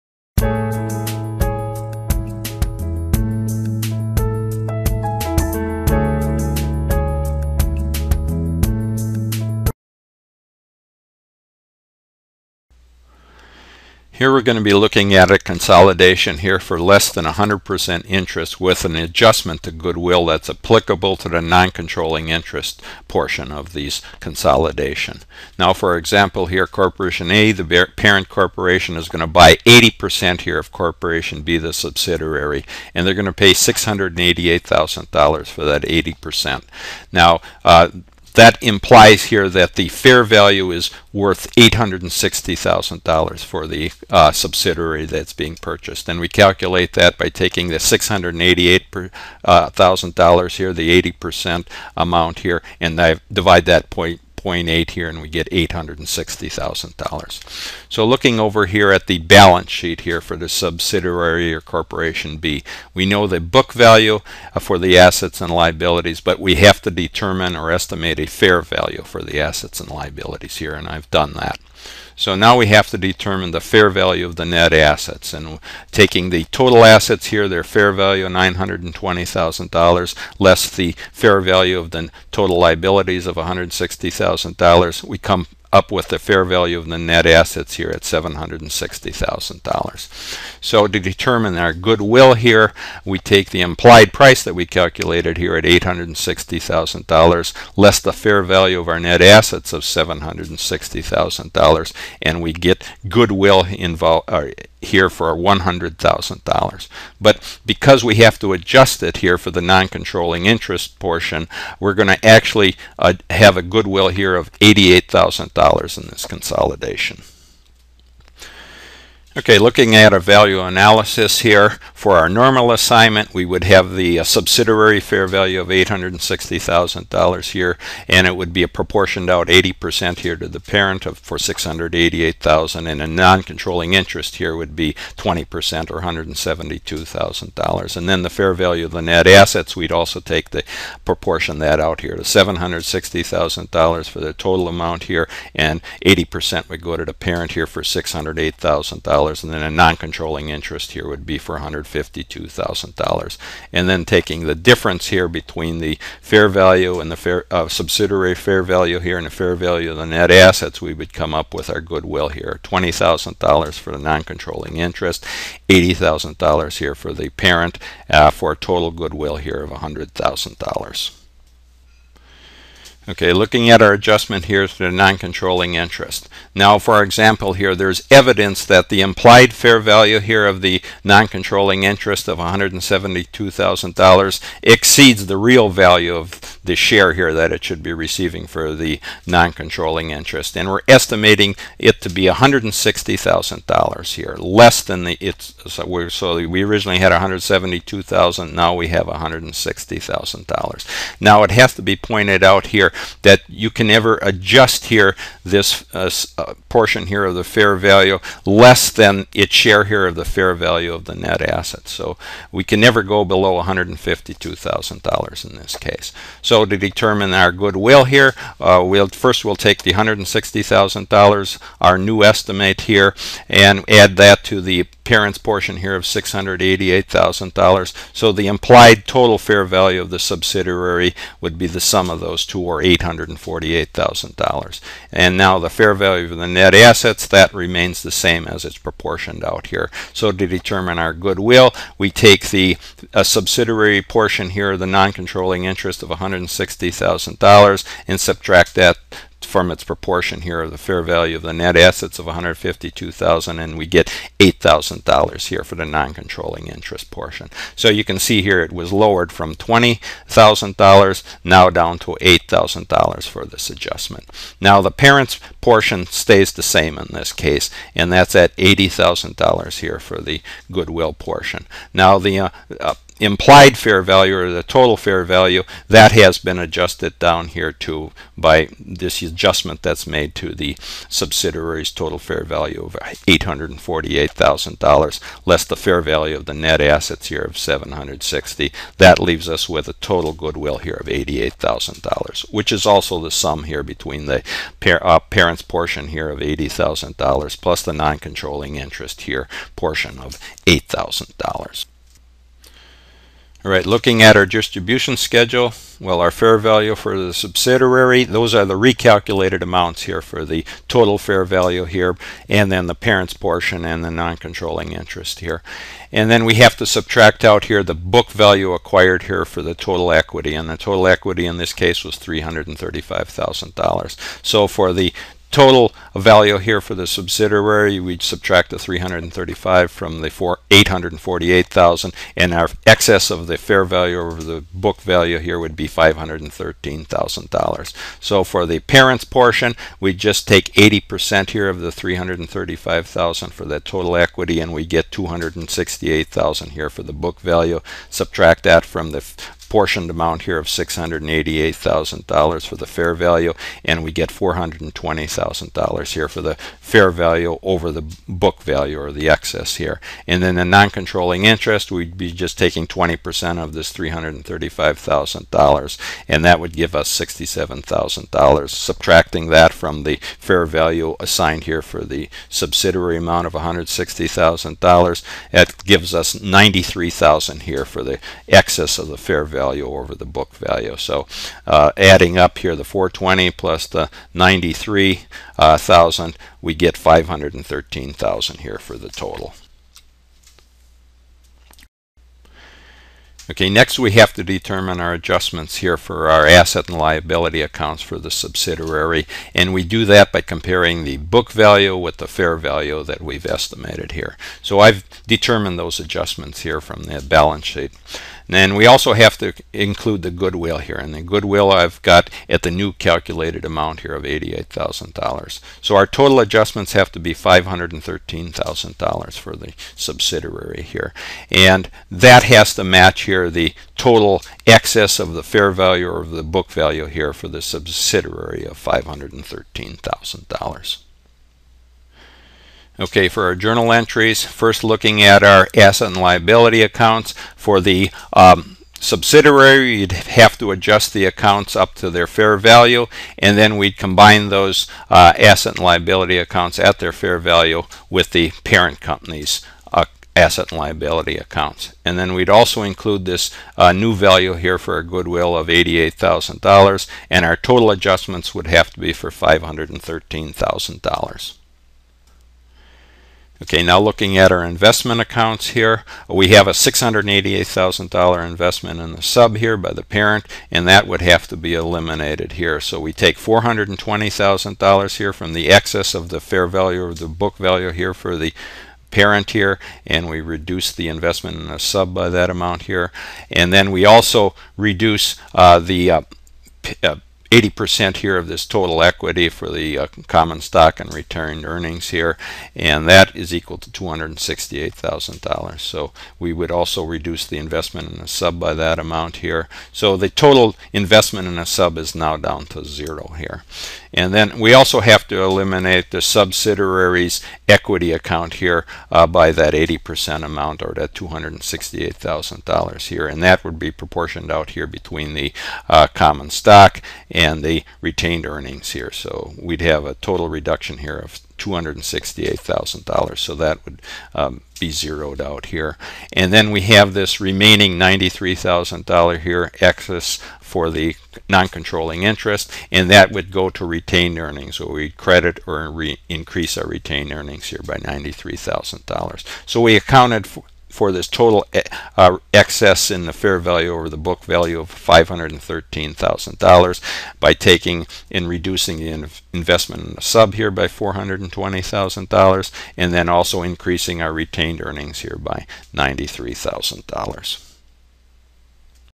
Goodwill Applied To Noncontrolling Interest For Business Consolidation
How to calculate goodwill in an acquisition, parent company purchases an interest in subsidiary and goodwill results in the acquisition, the goodwill is then divided between the parent and subsidiary companies, also included are the other accounts that have to be adjusted for the consolidation, book value versus fair value, detailed accounting example explained by Allen Mursau